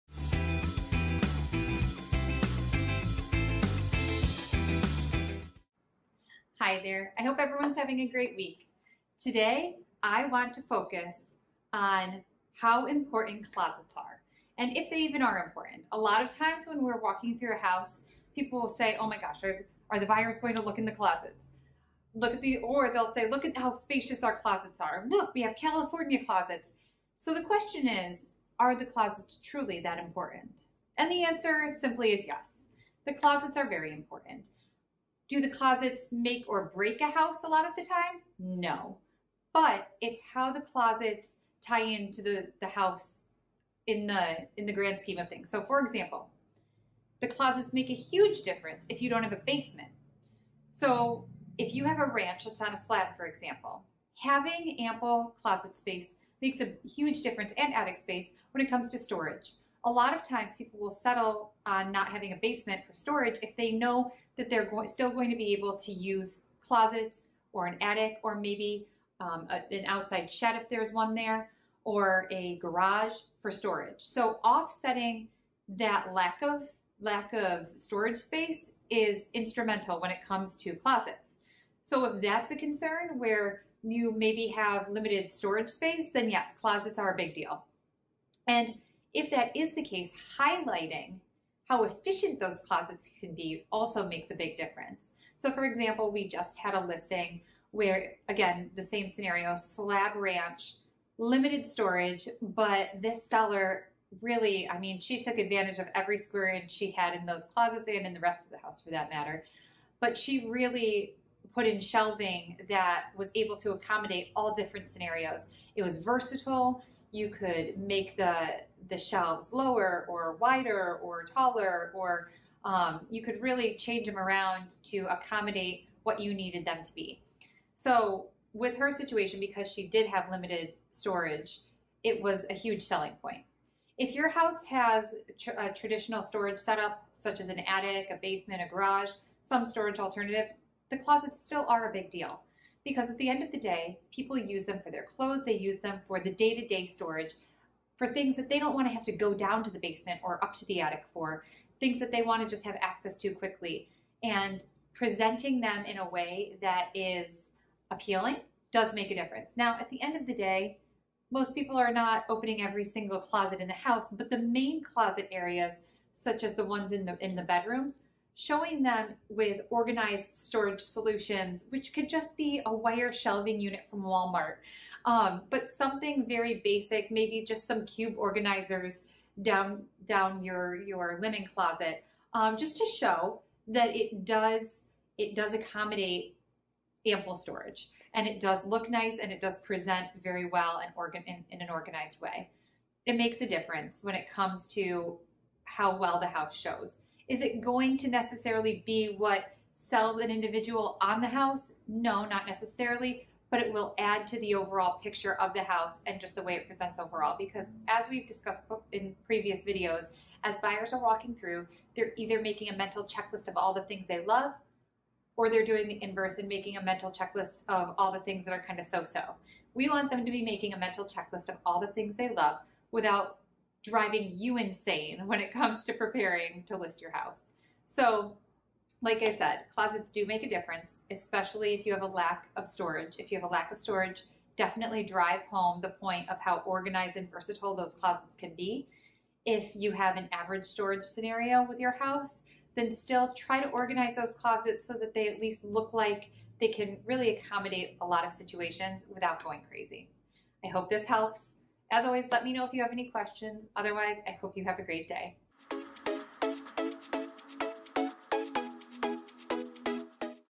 Are Closets Important?
Does closet space affect house sales? It all depends on the storage space in your home and how your closets are laid out. Jacky explains all the details in this video!